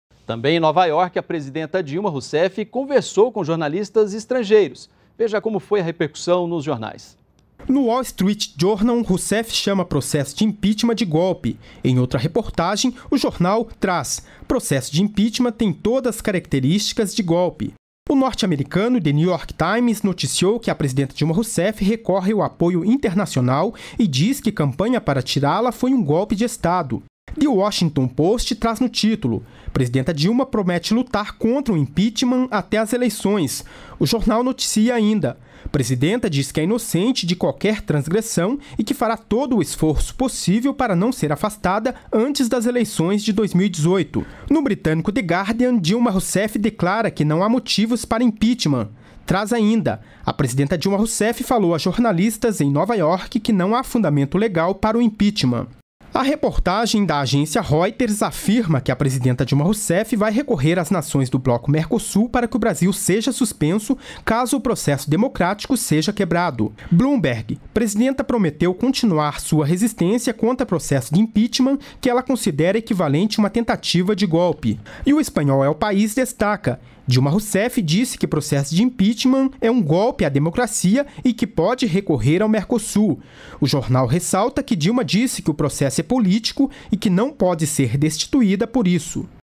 Jornais internacionais defendem mandato de Dilma Rousseff
NBR NOTÍCIAS - 25.04.16: A entrevista da presidenta Dilma Rousseff em Nova Iorque, na última sexta-feira (22), foi destaque nos principais jornais estrangeiros neste sábado (23). The Washington Post, The Guardian, The New York Times, entre outros veículos, publicaram sobre o momento político brasileiro e a defesa de Dilma.